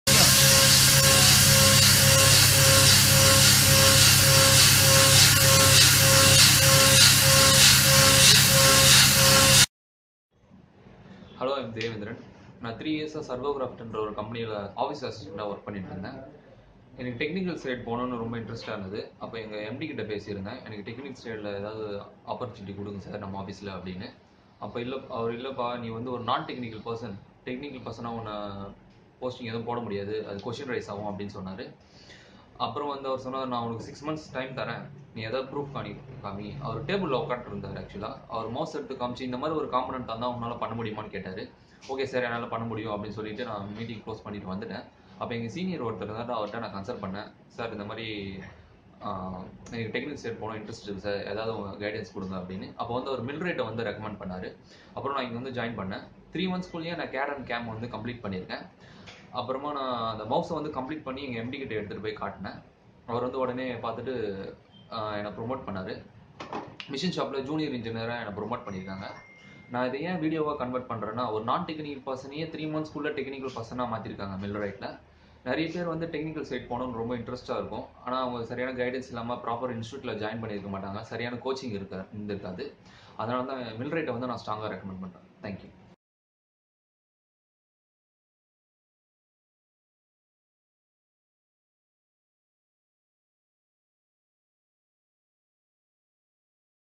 Devendiran changed to CNC Engineer from Office assistant, Mastercam training Millwright Hosur, India
An another feedback from Millwright Student ""Devendiran""